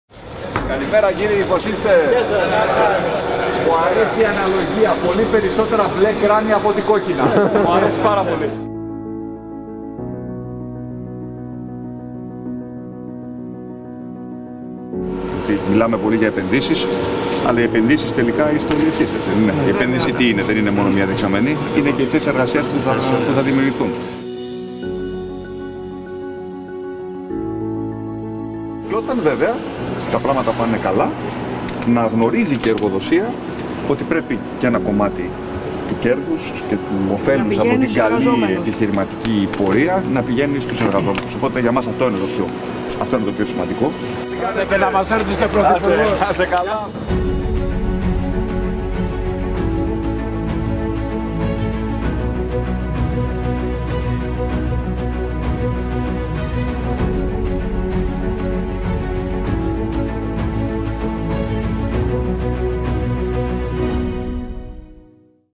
Ο Κυριάκος Μητσοτάκης στα Ναυπηγεία Χαλκίδας
Μιλάμε πολύ για επενδύσεις, γιατί οι πρώτοι ωφελημένοι είναι οι εργαζόμενοι. Διότι επένδυση τι είναι; Είναι οι νέες και καλά αμειβόμενες θέσεις εργασίας που θα δημιουργηθούν.